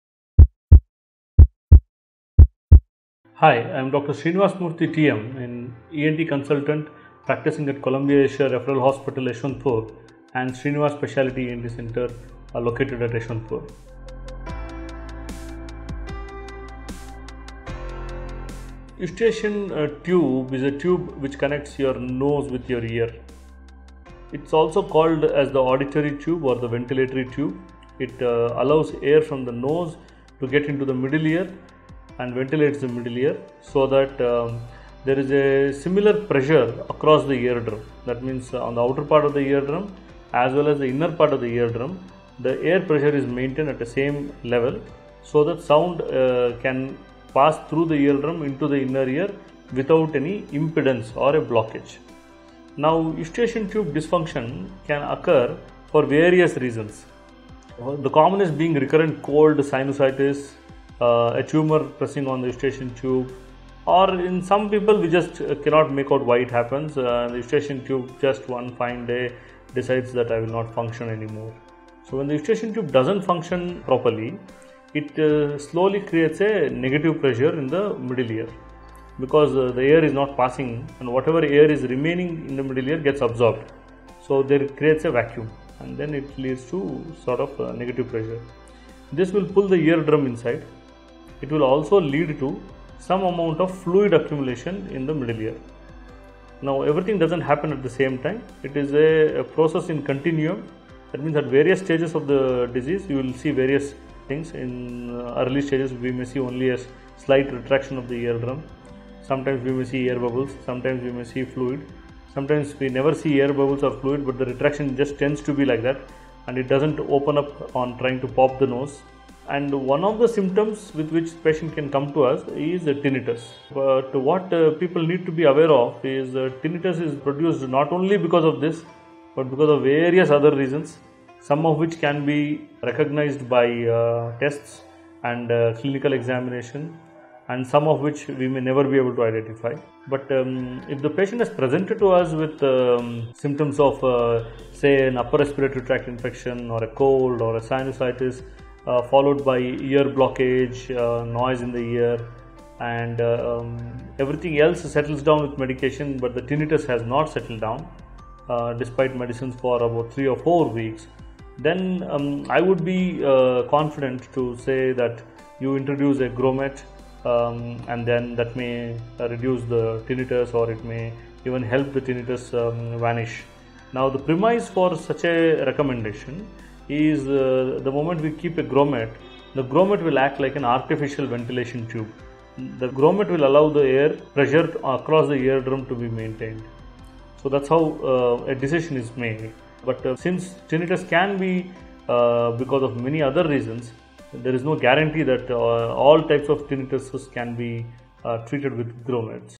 Do grommets help in curing Tinnitus caused due to ETD? - Dr. Sreenivasa Murthy T M
Now eustachian tube, is a tube which connects your nose with your ear. It is also called as the auditory tube or the ventilator tube. It allows the air from the nose to get int the middle ear and ventilates the middle ear, so  that there is a similar pressure across the eardrum, that means on the outer part of the eardrum as well as the inner part of the eardrum, the air pressure is maintained at the same level, so that sound can pass through the eardrum into the inner ear without any impedance or a blockage. Eustachian tube dysfunction can occur for various reasons, the commonest being recurrent cold, sinusitis, a tumor pressing on eustachian tube or in some people, we cannot just make out why it happens, eustachian tube just decides one fine day that  will not function any more. So when the eustachian tube doesn’t function properly, it slowly creates a negative pressure in the middle ear, because if the air is not passing and whatever air is remaining in the middle ear is gets absorbed. So that creates a vaccum and it leads to sort of negative pressure. This will pull the eardrum inside. It will also lead to some amount of fluid accumulation in the middle ear. Now, everything doesn’t happen  at the same time. It is a process in continuum, that means at various stages of the disease, you will see various things and , and early stage we may see as slight retraction of the eardrum, sometimes we will see air bubbles, sometimes we may see fluid, sometimes we never see air bubbles or fluid, but the retraction just tends to be like that  and it doesn’t open up on trying to pop the nose, and one of the symptoms with which the patient can come to us with tinnitus. To what people need to be aware of is  a tinnitus is produced not only because of this, but because of various other reasons, some of which can be  recognized by tests and clinical examination, and some of which we may ever be able to identify, but if the patient is presented to us ,  symptoms of say upper respiratory tract infection or a cold or a sinusitis, followed by ear  blockage, noise in the ear  and everything else settles down with medication, but  the tinnitus has not settled down, despite medications for 2 or 3 weeks, then I would be confident to say that you introduce a grommet  and that they may reduce the tinnitus or it may help the tinnitus to vanish. Now the premise for such a recommendation is the moment we keep a grommet, the grommet will act like an artificial ventilation tube.  The grommet will allow the air pressure across the eardrum to be maintained. So that is how a decision is made, but since tinnitus can be because of many other reasons, there is no guarantee that all types of tinnitus can be treated with grommets.